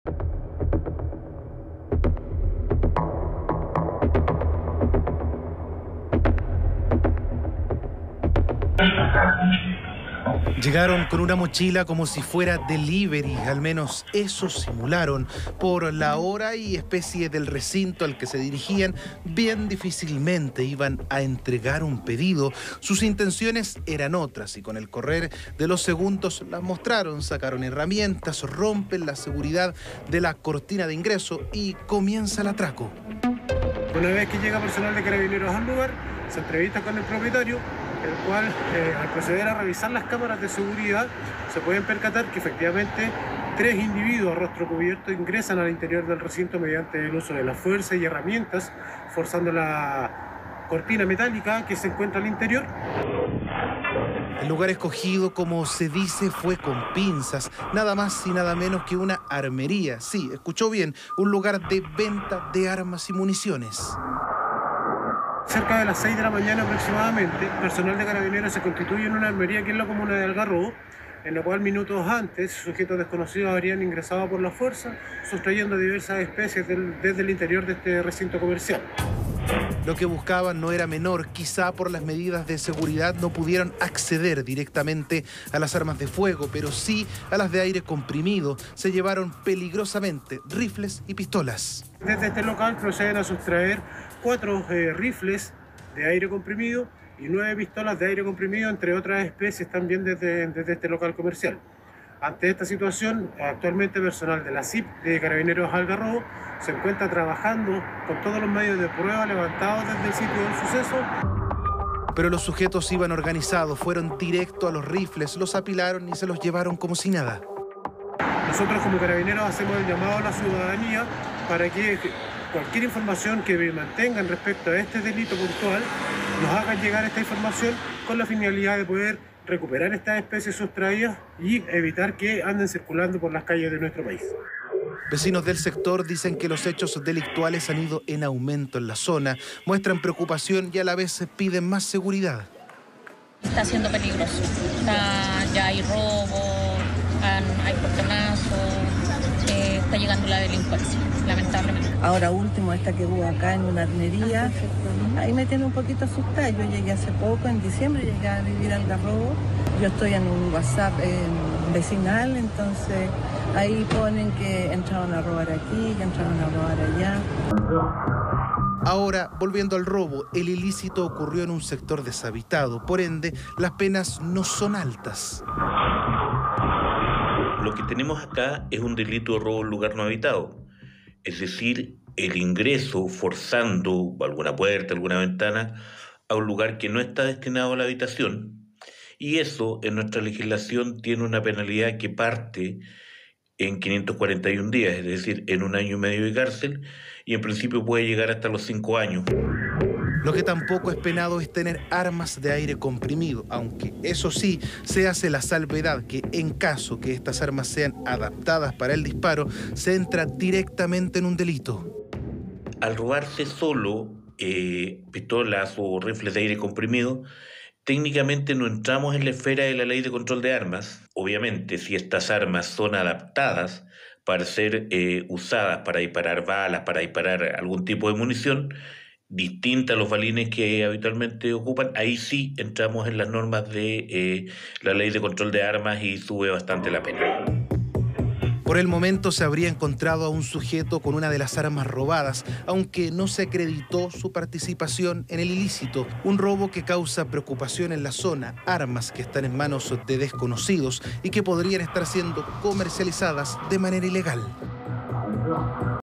Delincuentes roban armería en Algarrobo: Lograron sustraer armamento de aire comprimido
Un impactante robo sucedió en una armería en Algarrobo, Región de Valparaíso. Los delincuentes lograron robar variedad de armamento de aire comprimido, arcos de deporte y munición.